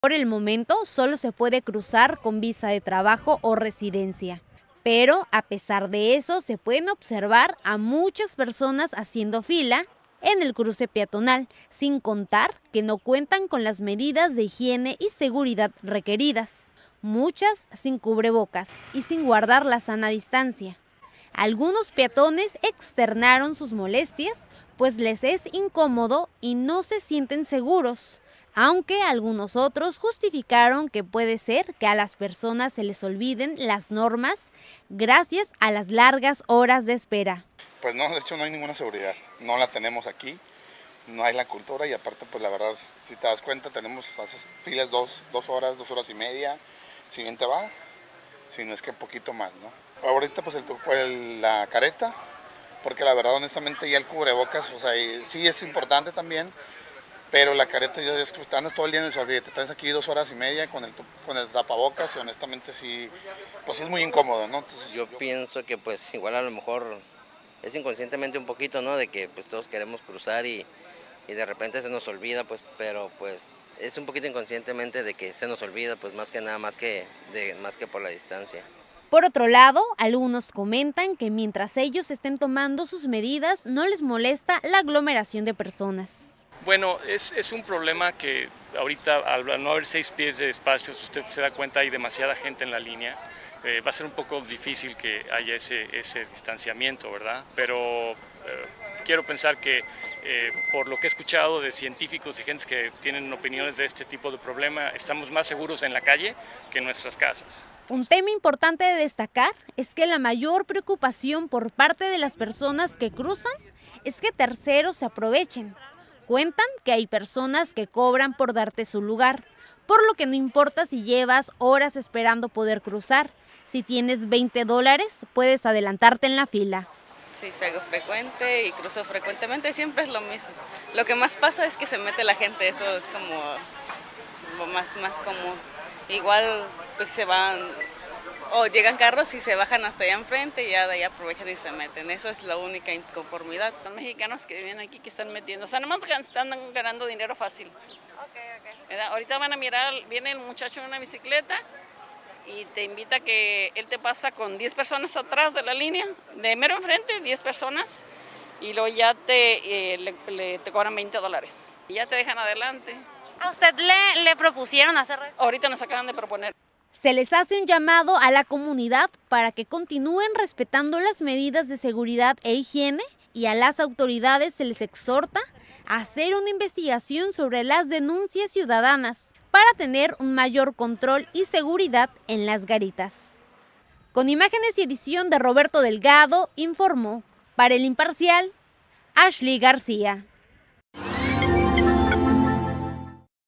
En la Garita de Otay no se están respetando las medidas de seguridad e higuiene.
Las Garitas, un foco de infección de Covid-19.